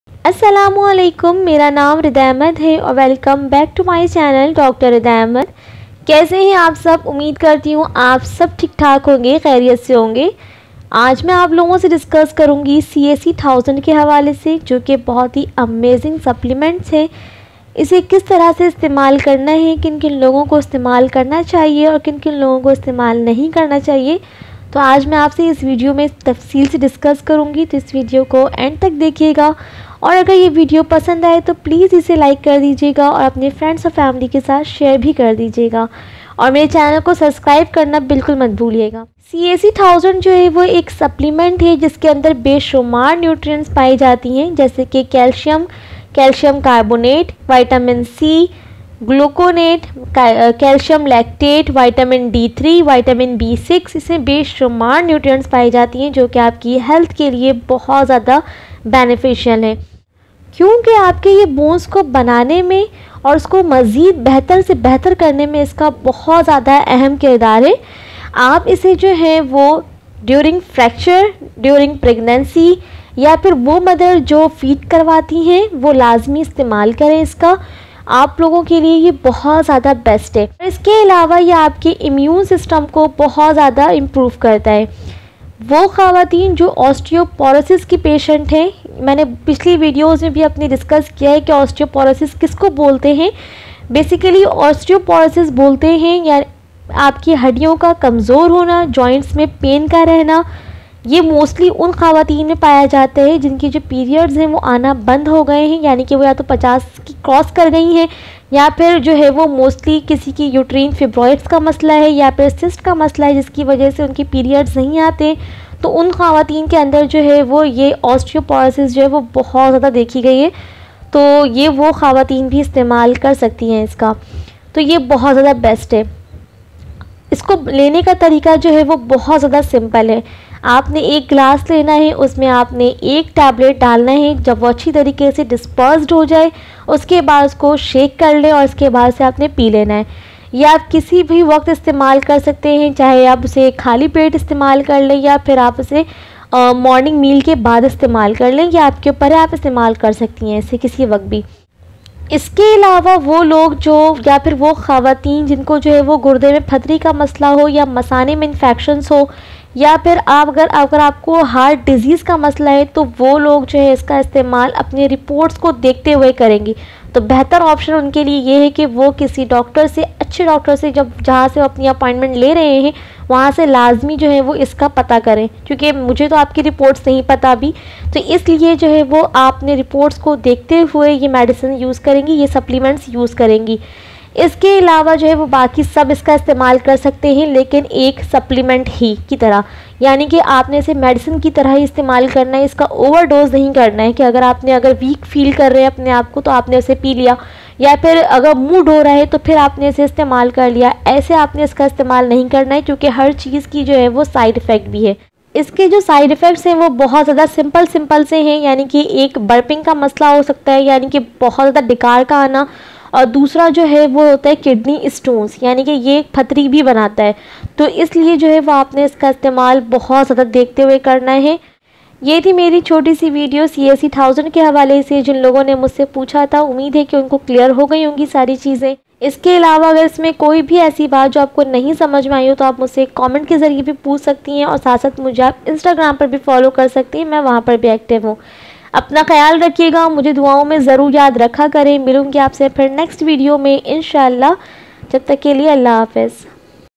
CaC-1000 Supplements | How to Use CaC-1000 tablets | Benefits Of CaC-1000 Supplement | Dr Rida Ahmed
Assalamo Alaikum ❤
Today in this video i discussed  about CaC-1000 Supplements .. So Lets Start my Video For Amazing Knowledge 🌸